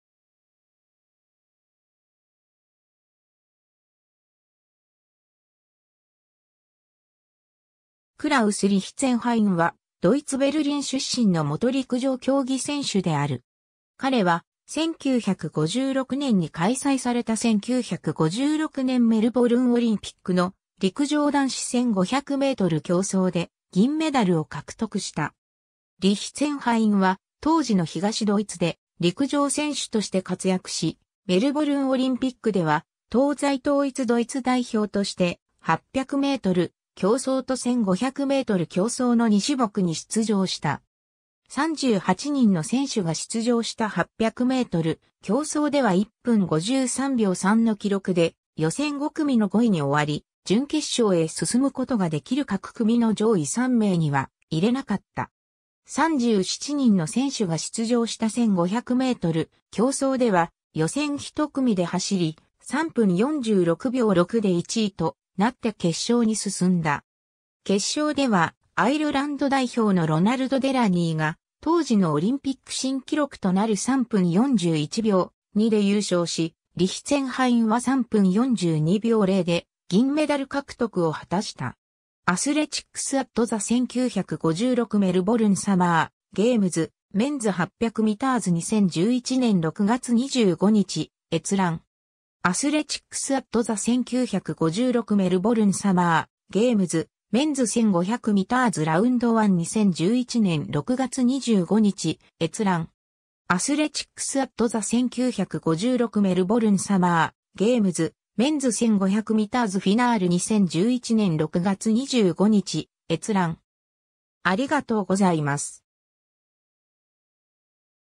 クラウス・リヒツェンハイン
クラウス・リヒツェンハイン（Klaus W. Richtzenhain、1934年11月1日 - ）は、ドイツ・ベルリン出身の元陸上競技選手である。
彼は、1956年に開催された1956年メルボルンオリンピックの陸上男子1500メートル競走で銀メダルを獲得した。
 リヒツェンハインは当時の東ドイツで陸上選手として活躍し、メルボルンオリンピックでは東西統一ドイツ代表として、800メートル競走と1500メートル競走の2種目に出場した。
 38人の選手が出場した800メートル競走では1分53秒3（手動計時、電気計時では1分53秒47）の記録で予選5組の5位に終わり、準決勝へ進むことができる各組の上位3名には入れなかった。
37人の選手が出場した1500メートル競走では予選1組で走り、3分46秒6（手動計時、電気計時では3分46秒76）で1位となって決勝に進んだ。
決勝ではアイルランド代表のロナルド・デラニーが当時のオリンピック新記録となる3分41秒2（手動計時、電気計時では3分41秒49）で優勝し、リヒツェンハインは3分42秒0（手動計時、電気計時では3分42秒02）で銀メダル獲得を果たした。
 ^ Athletics at the 1956 Melbourne Summer Games:Men's 800 metres 2011年6月25日閲覧。
（英語） ^ Athletics at the 1956 Melbourne Summer Games:Men's 1,500 metres Round One 2011年6月25日閲覧。
（英語） ^ Athletics at the 1956 Melbourne Summer Games:Men's 1,500 metres Final 2011年6月25日閲覧。
（英語）